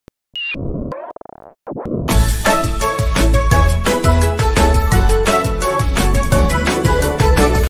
[YTP TENNIS] RaymanChrome's TV Grid, a greenpizzagrease's Trail Wheel? I don't give a damn.
This is round 123/200 of @TheCuddlesStudios's 10 Way 2 Seconds Tennis.

--My Laptop Equipment (updated 10th apr. 2022)--